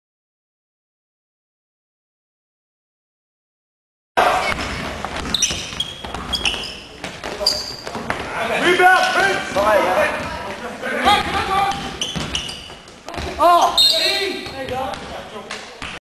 Zain Nadeem getting to the foul line
Zain Nadeem getting to the foul line against the Titans